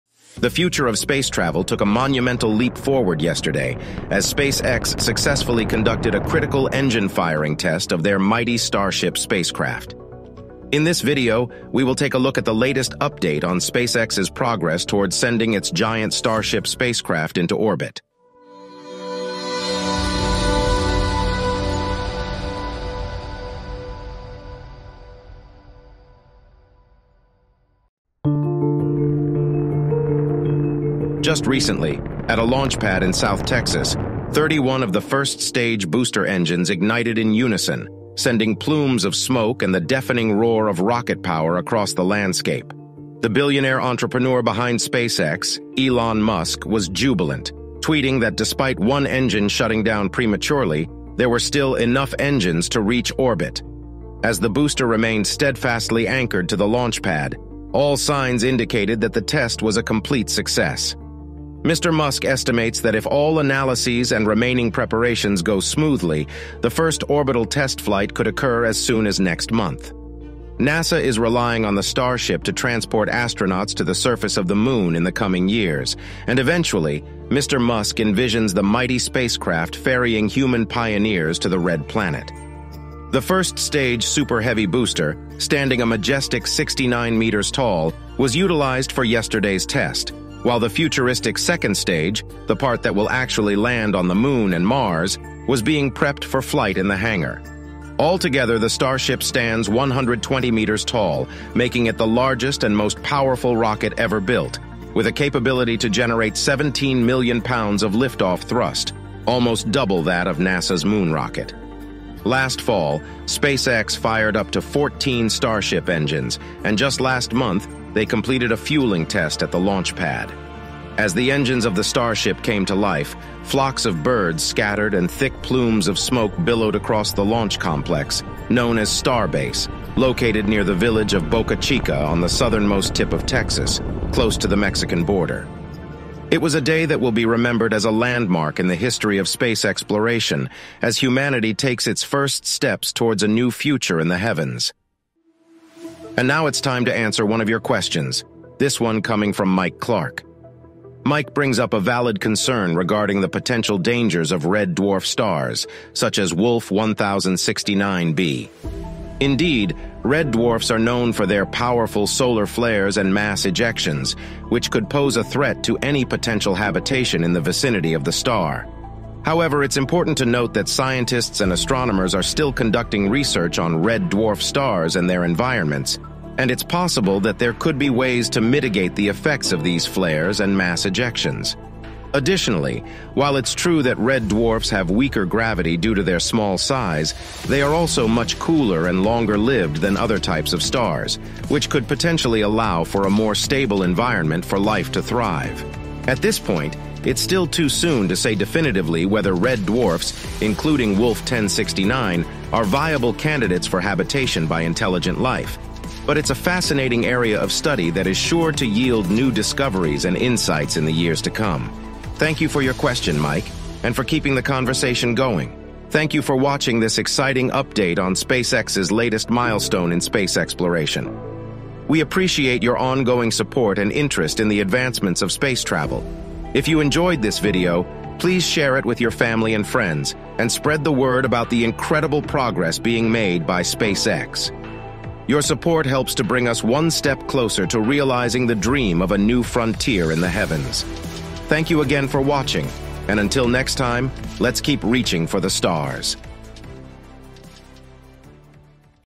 Blast Off! SpaceX Takes a Giant Leap Toward Launching its Starship to Orbit
Get ready for an exciting journey into space! SpaceX has taken a major step towards launching its giant starship spacecraft into orbit with a successful engine-firing test. Follow SpaceX's mission to send astronauts to the moon and beyond, led by visionary CEO Elon Musk. Discover the potential for human life on other planets and the future of space travel. Read on for all the details!